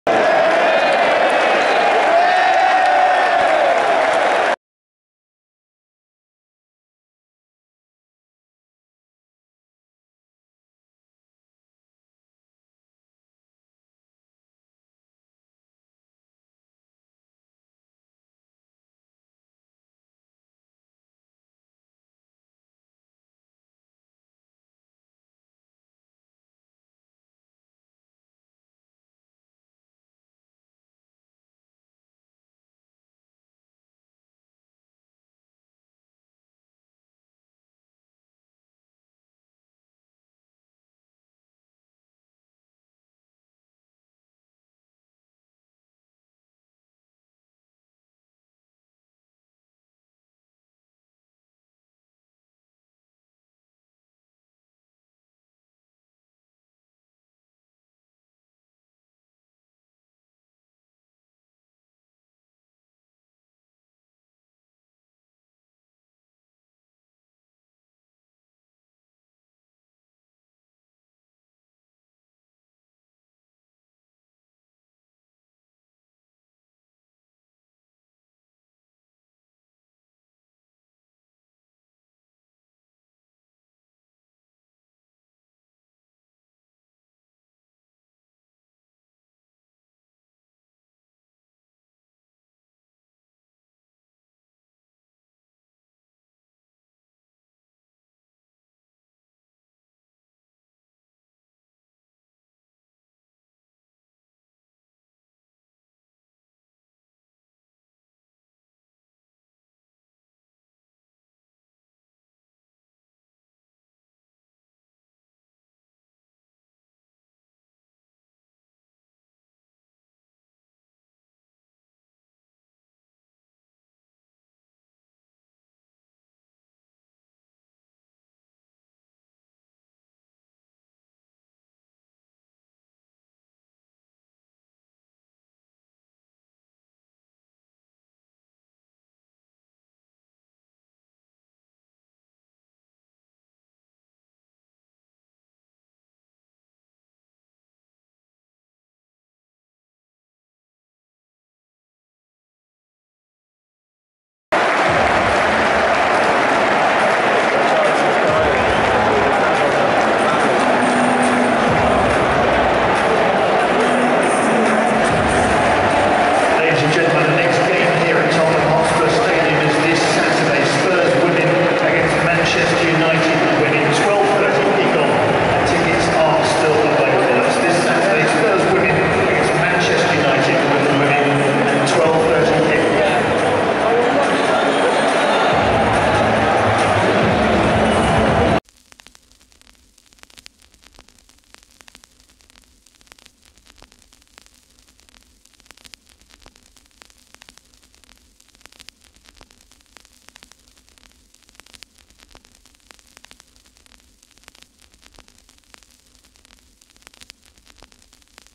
경기 끝난 뒤 손흥민 안아주는 콘테 감독,토트넘 동료들과 포옹,상대편 선수들과 인사 20220907 챔피언스리그 조별예선 토트넘:마르세유
경기 끝난 뒤 손흥민과 포옹하는 콘테 감독,토트넘 선수들
상대편 선수들과 인사

20220907
챔피언스리그 조별예선
토트넘:마르세유
토트넘 핫스퍼 스타디움